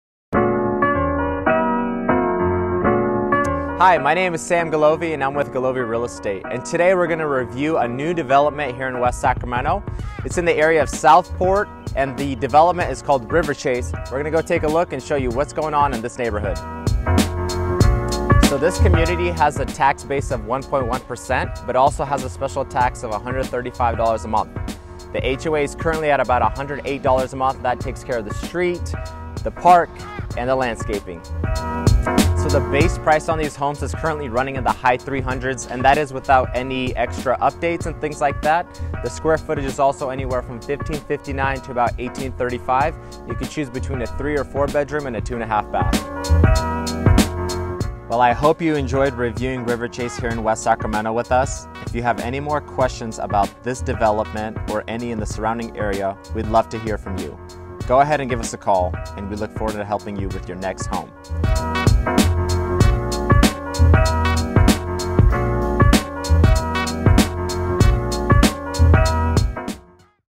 New Homes in West Sacramento - Riverchase at SouthPort
Riverchase in West Sacramento is offering some affordable newly built options that is sure to be the epitome of convenience for you and your family!